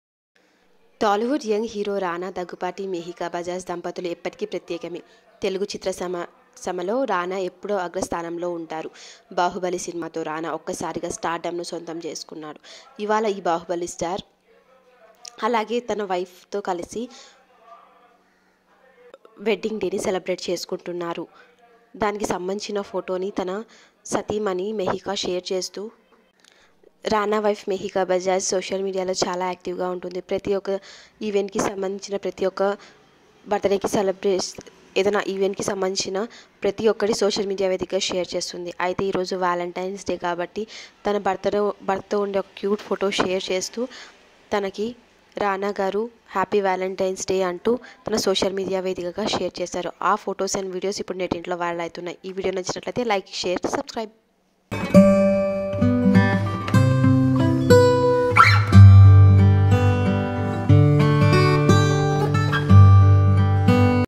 Rana Wife Miheeka Bajaj Emotional Post on Social Media On Valentine's day/ Rana # Miheeka
Rana Miheeka Bajaj shares a cute pictures